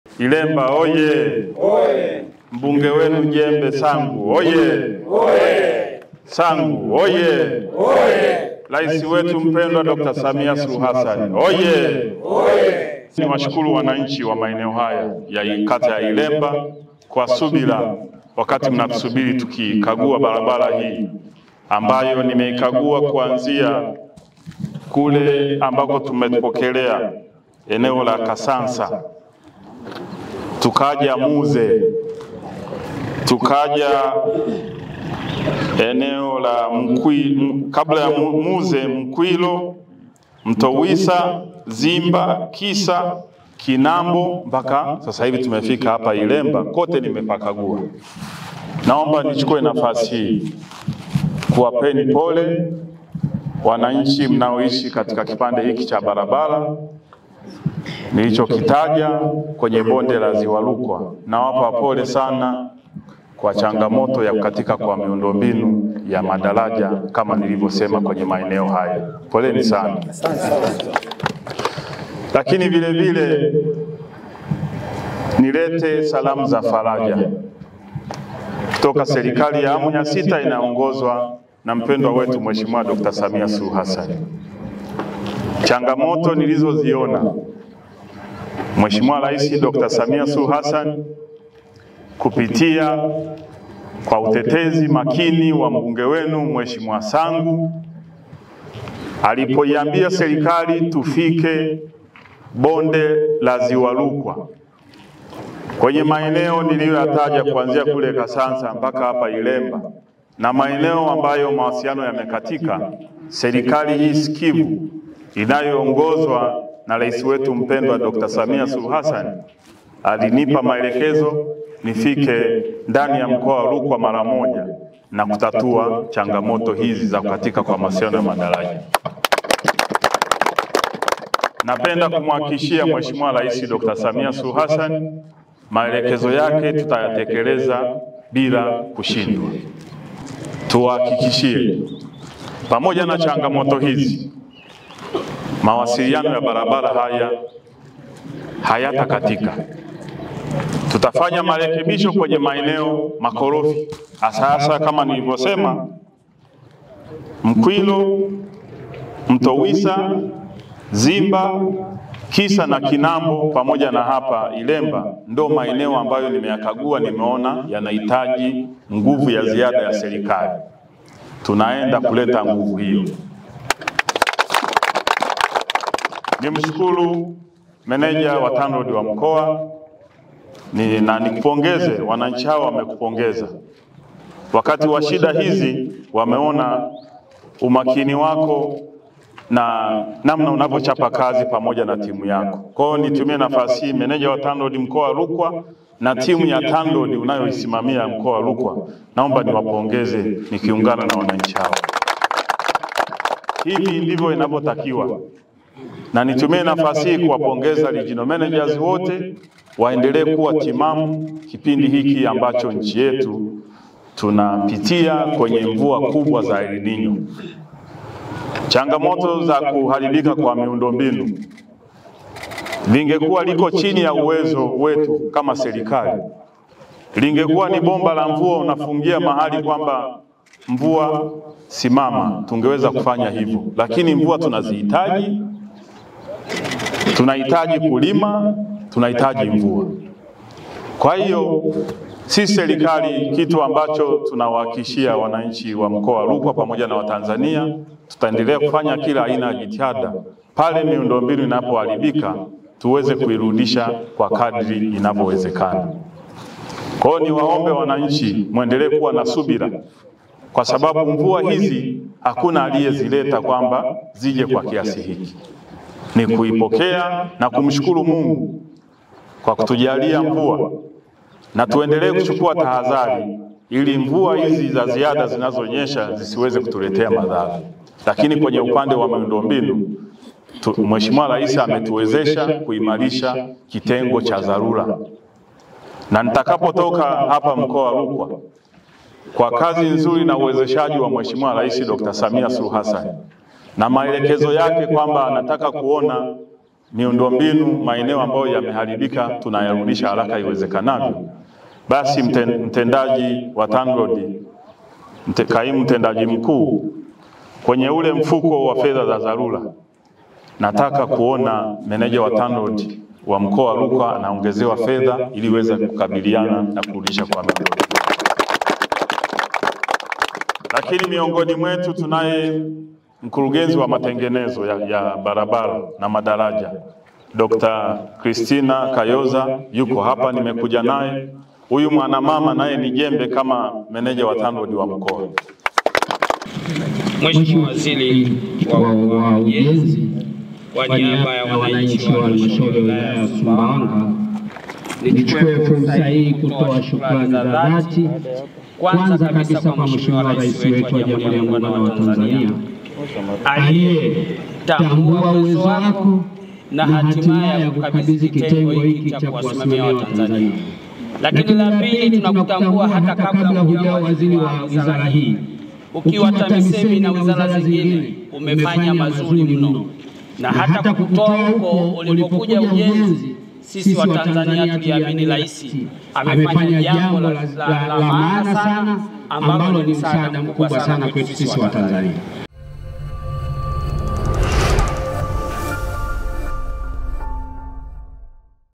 MIUNDOMBINU YA BARABARA ILIYOATHIRIWA NA MVUA RUKWA KUREKEBISHWA
Waziri wa Ujenzi, Innocent Bashungwa amesema Serikali imejipanga kuhakikisha inarejesha miundombinu ya barabara ya Kasansa - Muze - Mtowisa - Ilemba - Kaoze - Kilyamatundu (km 180) iliyoathiriwa kwa kiasi kikubwa na mvua zinazoendelea kunyesha maeneo tofauti tofauti nchini.

Bashungwa ameeleza hayo Aprili 08, 2024 Mkoani Rukwa wakati akikagua barabara hiyo na kuwahakikishia wananchi kuwa Serikali itafanya  matengenezo makubwa katika maeneo korofi ya Nkwilo, Mtowisa, Zimba,  Kisa, Kinambo, Ilemba na Solola ambayo yameathiriwa kwa kiasi kikubwa.